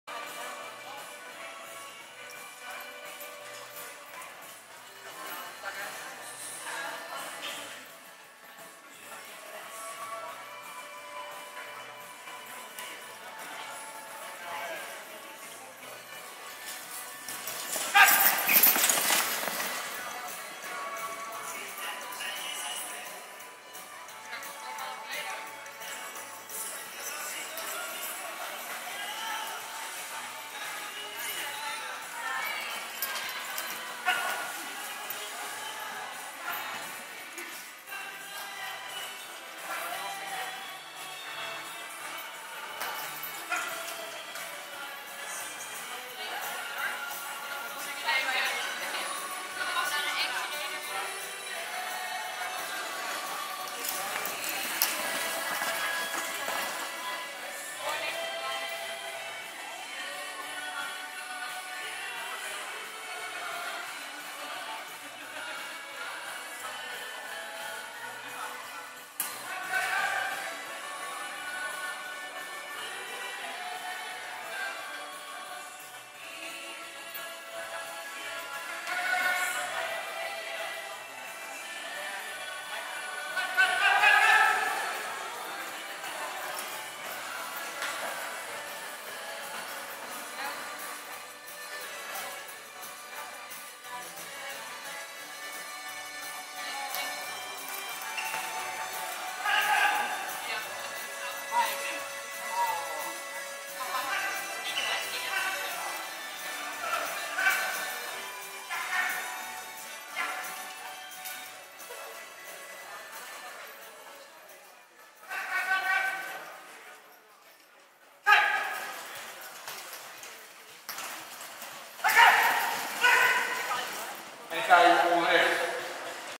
Fahrturnier Denekamp (Twente Cup) 12.11.2022
Ein kurzes Video einer Kegel-Fahr-Prüfung mit festen Gelände Hindernissen vom Fahrturnier in Denekamp
Twente Cup